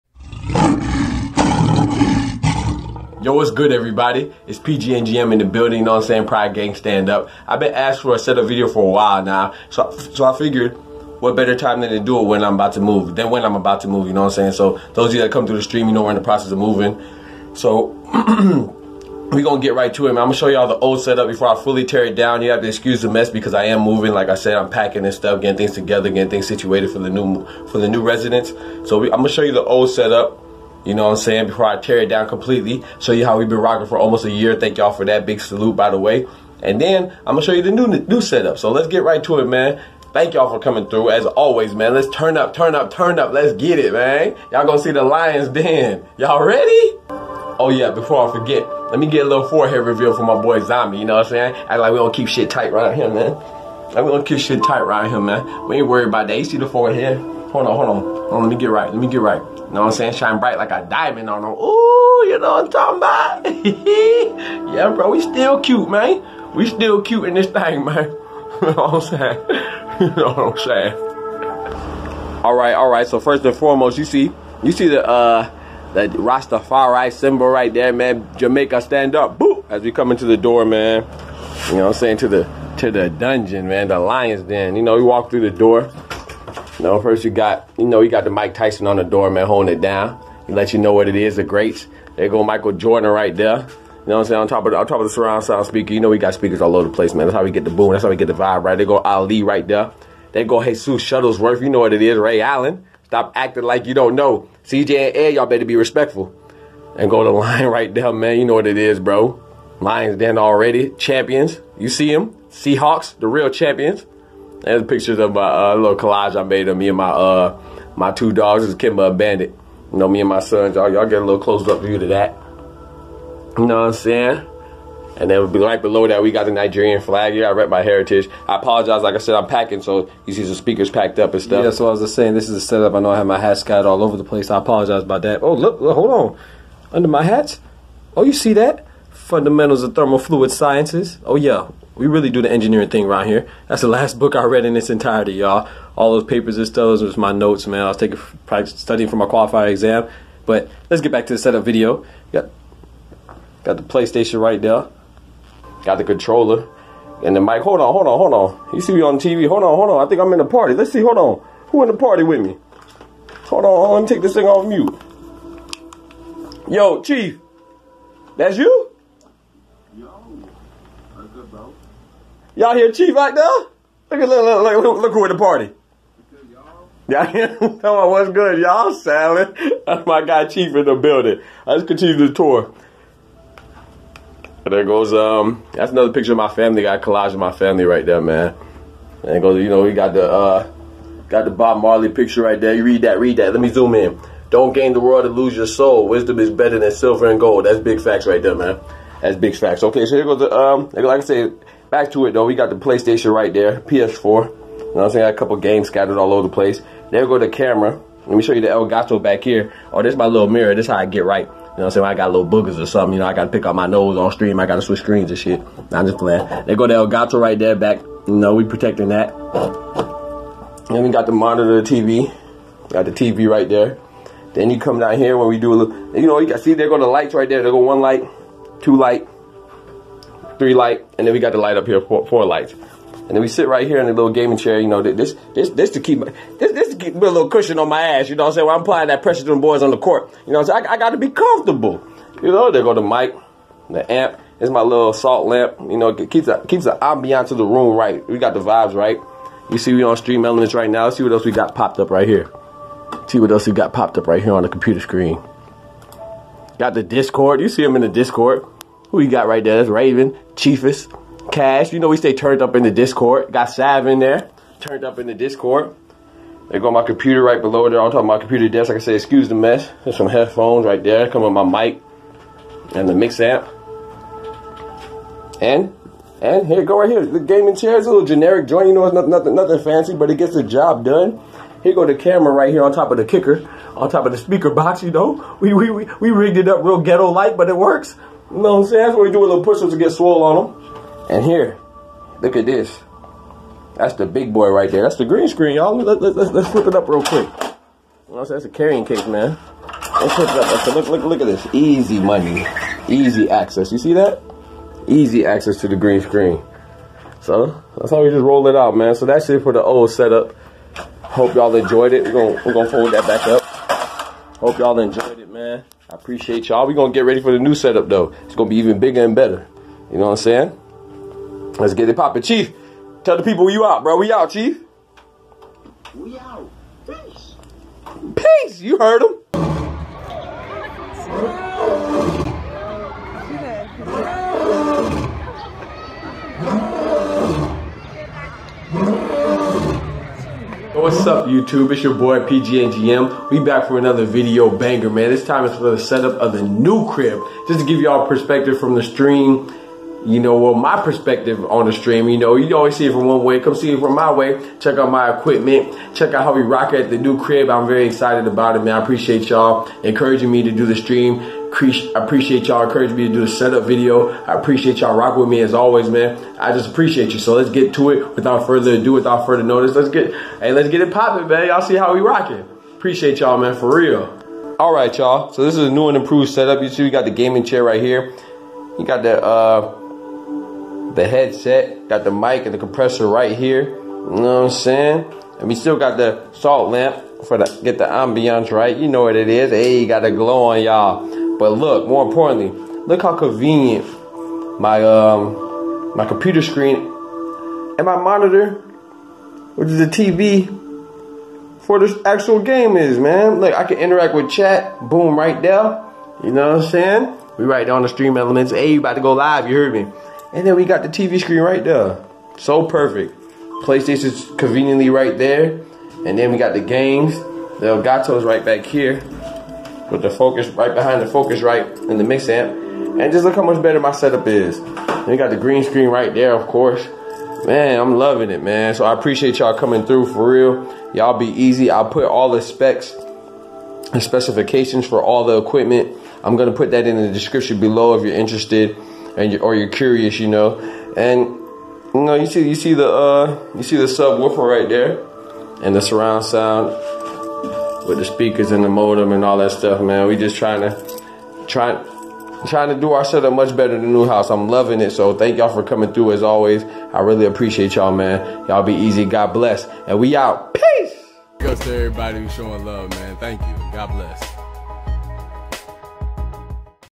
Best Gaming Setup for Streamers 2020, Ultimate, Inexpensive and Affordable, Game Like A Pro!
Best Gaming Setup for Streamers 2019, Ultimate and Affordable, Game Like A Pro!

Thanks for the love, Jah bless!

Below are the specifications...

Setup Components (w/ Prices):

Computer – HP Pavilion Desktop ($1000)
• OS – 64-bit, Windows 10
• Processor – i7 (Intel, 8th Generation)
• GFX Card – Intel UHD 630 (Keep in mind that I game off of console, not PC, this is just the interface between console and Elgato)

Console – Playstation 4 ($350)

Game Capture Device – Elgato HD60 S ($150)

Primary (Front) Webcam – Logitech C920 HD Pro ($40)

Secondary (Side) Webcam – Logitech QuickCam S5500 (idk, but cheap)

Mic – Focusrite Scarlett Solo (2nd Generation) Studio Bundle ($250)

Game Monitor – 36 inch (32 inch screen width) Vizio Television ($300)

Computer Monitor – Dell S2415H Color Profile, D6500 (Gift from Professor)

Headset – HyperX Cloud Stinger ($55)

Surround Sound – Logitech X-530 (idk)

Gaming Chair – Generic As S**T, but PERFECT for me! (Gift from Loving Mother)

BEST GAMING SETUP, BEST GAMING EQUIPMENT, NEW STREAMERS, MUST HAVE FOR STREAMERS, STREAMING EQUIPMENT, ELGATO GAMING